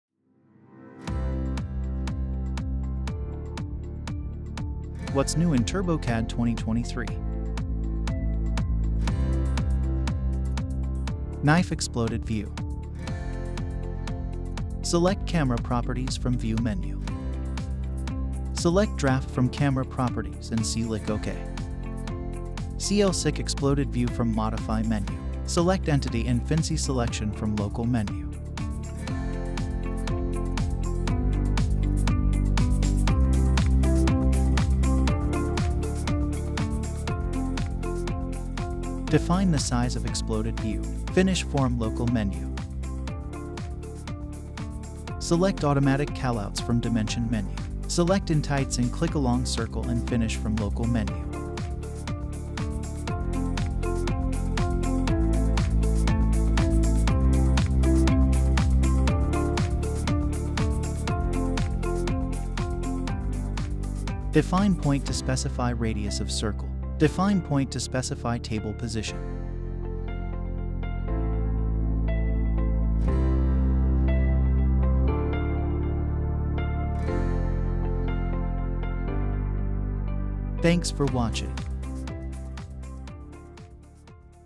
Knife Exploded View
Select Camera Properties from View Menu
Select Draft from Camera properties and Click OK
Click Exploded View From Modify Menu
Select Entity and Finish Selection from Local Menu.
Define the size of Exploded View
Finish Form Local Menu
Select Automatic Callouts from Dimension Menu
Select entites and Click along circle and Finish from Local Menu
Define point to specify radius of circle
Define point to specify table position